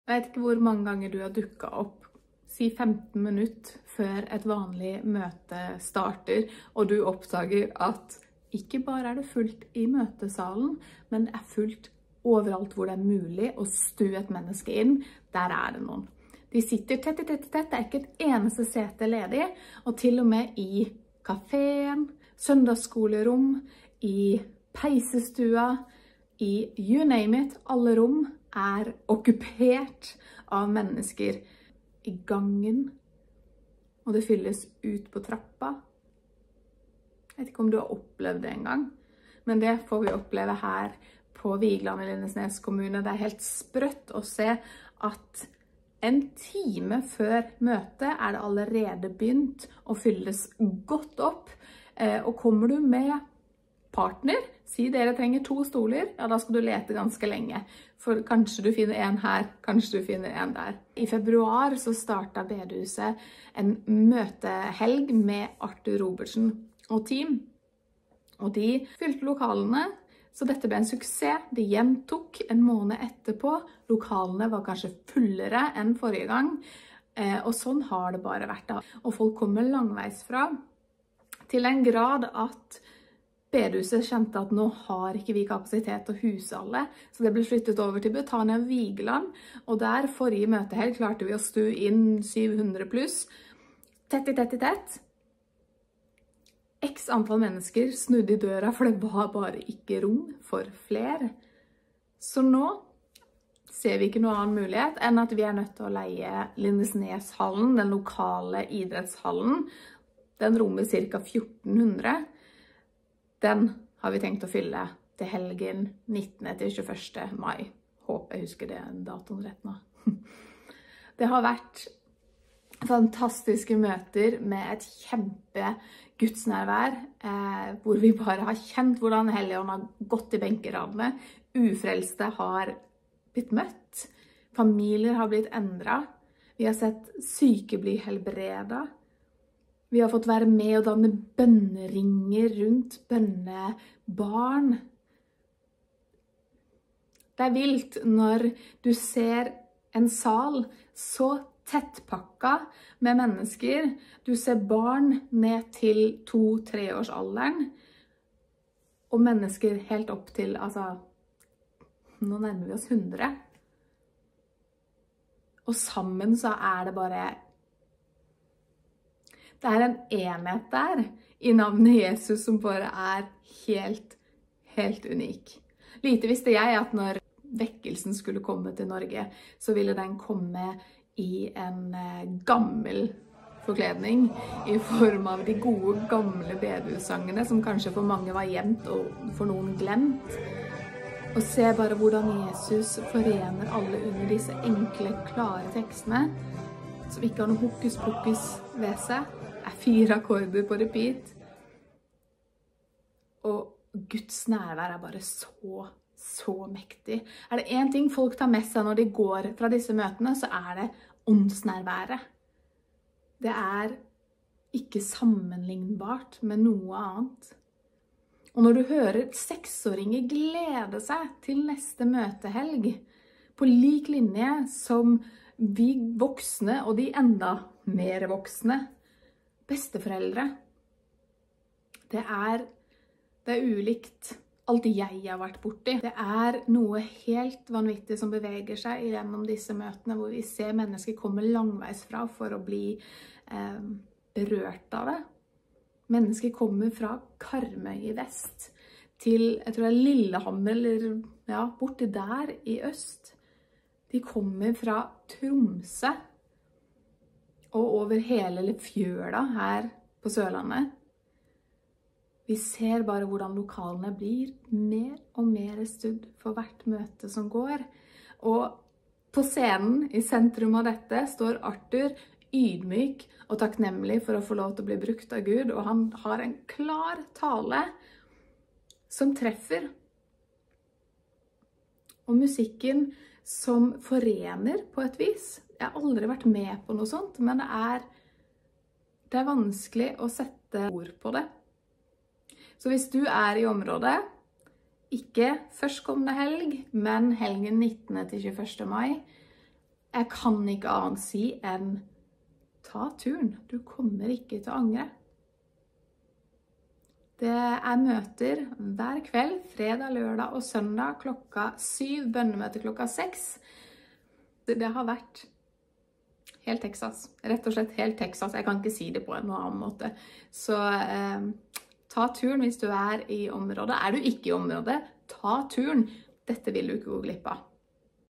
Vekkelsen på Vigeland- Nina Slotten-Lauvsland forteller sin opplevelse av vekkelsen som bor der fast
Vekkelse på Vigeland!

Jeg er så heldig at jeg har en søster på Vigeland som får oppleve vekkelsen på nært hold. Jeg spurte henne om hun ville dele hvordan hun opplever vekkelsen, som bor på Vigeland og kjenner godt til menighetene og folket som bor der fast.